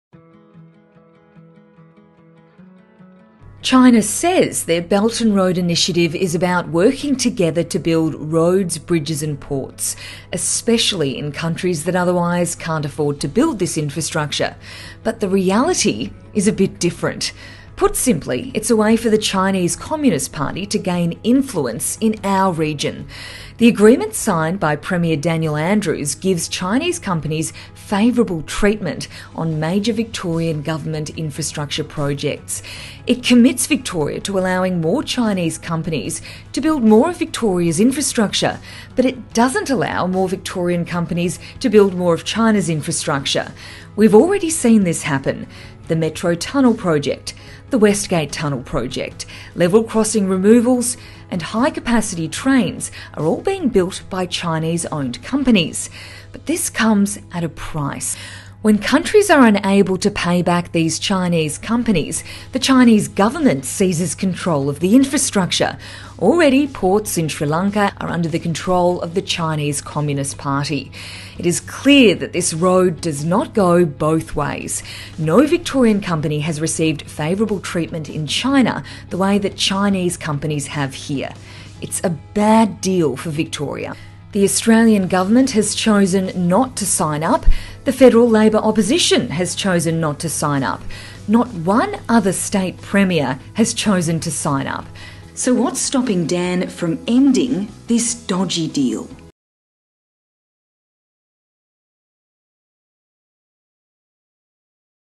Why Daniel Andrews' Belt and Road deal is bad for Victoria
This morning I have received hundreds of messages and questions about why Daniel Andrews' Belt and Road deal is bad for Victoria.

Check out and share this video explainer on Victoria’s Belt and Road deal, a deal that Labor signed us up to, that was a dud deal for Victoria and a dud deal for Victorian jobs.